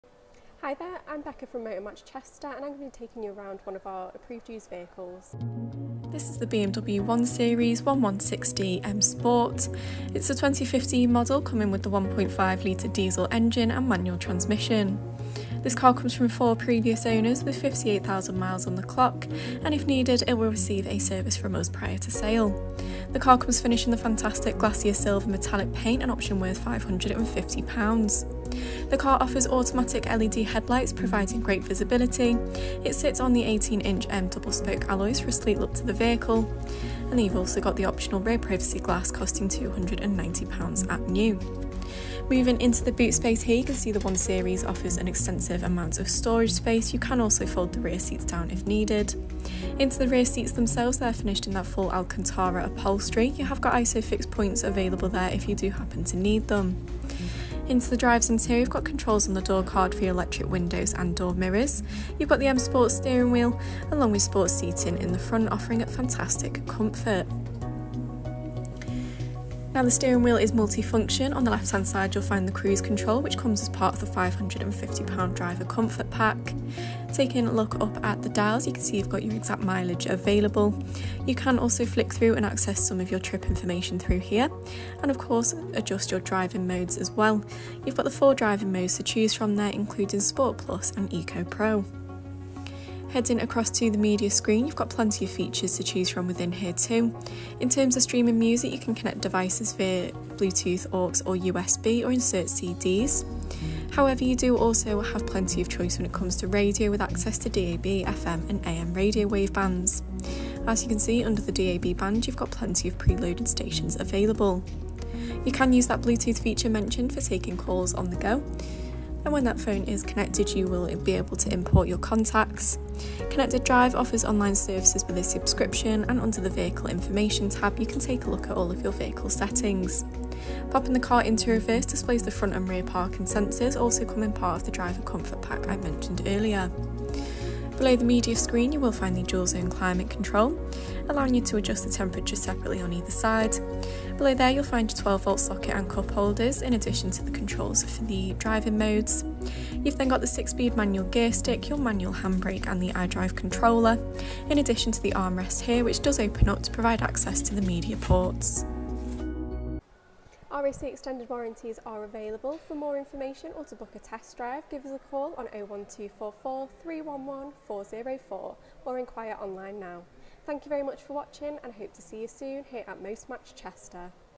Used BMW 1 Series 1.5 116d M Sport | Motor Match Chester
Just into our stock, this 2015 BMW 1 Series 1.5 116d M Sport in Glacier Silver is now available for sale from Chester.
Reg number: YB65 FTE
Fuel: 1.5L Diesel
Transmission: Manual
Mileage: 58,429 miles

Some top features on this car include:
Bluetooth interface
Cruise control